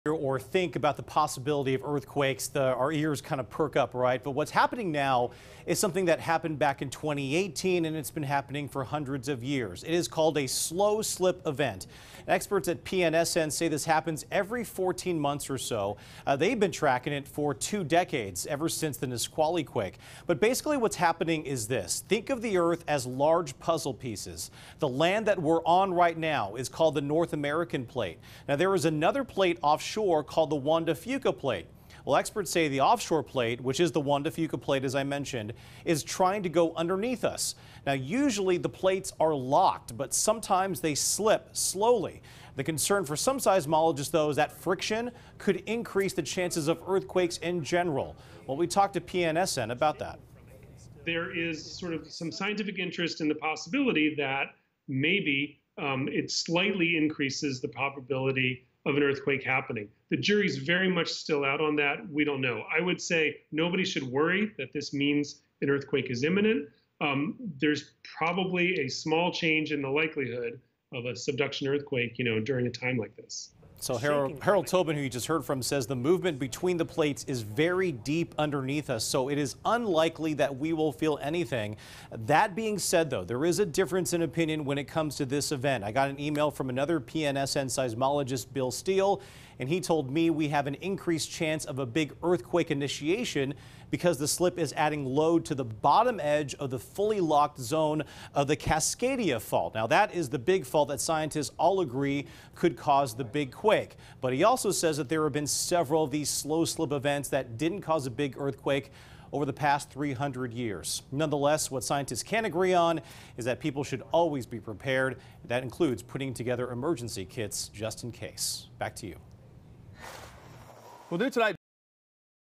Seismic slow-slip event happening now, but will it increase the chance of an earthquake?
Seismologists are monitoring a seismic event that they say is happening right now. Its called a slow-slip event. According to the Pacific Northwest Seismic Network, this event happens about every 14 months.

PNSN has been tracking it for about two decades, ever since the Nisqually earthquake